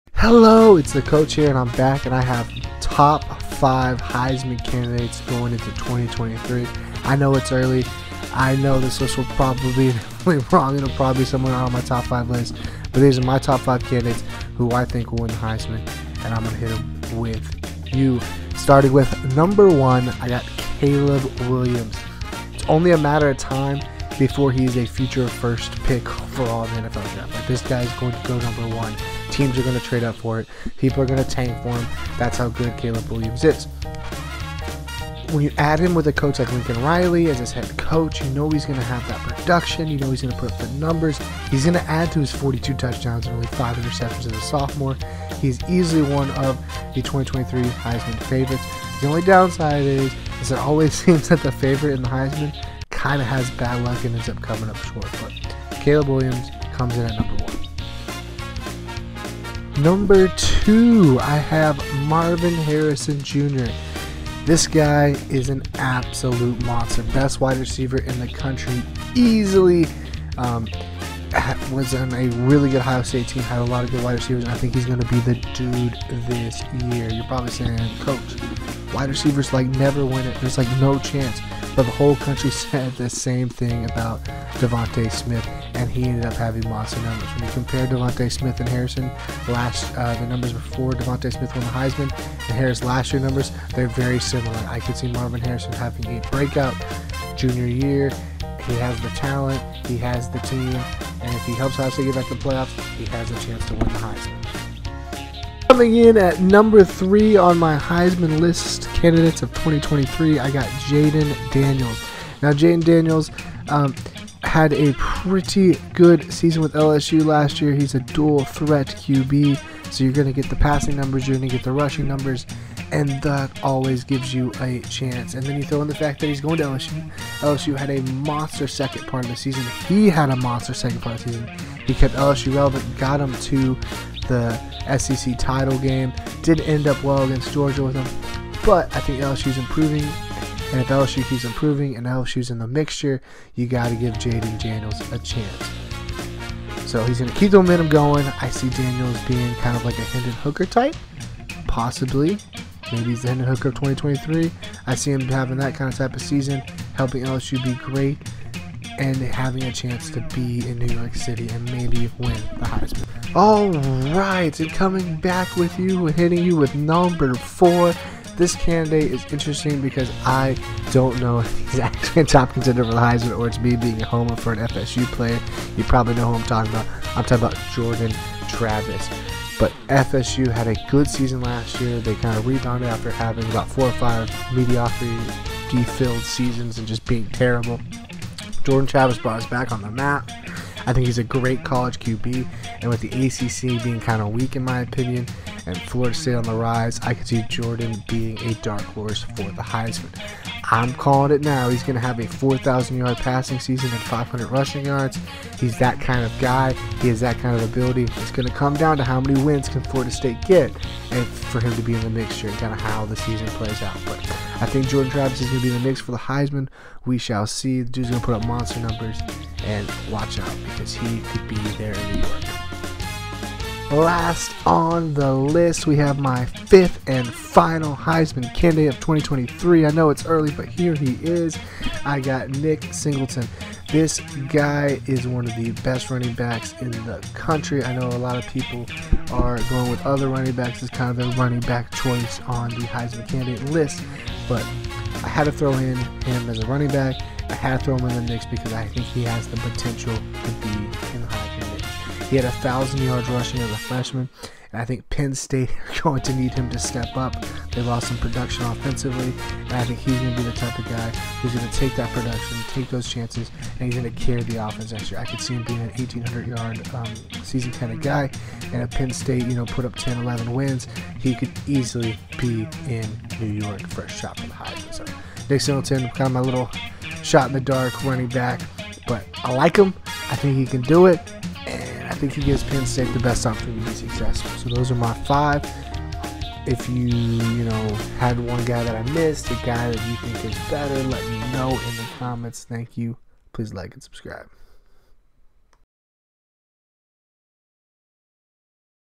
5 Heisman Candidates for 2023
on this video the coach talks about his 5 candidates for 2023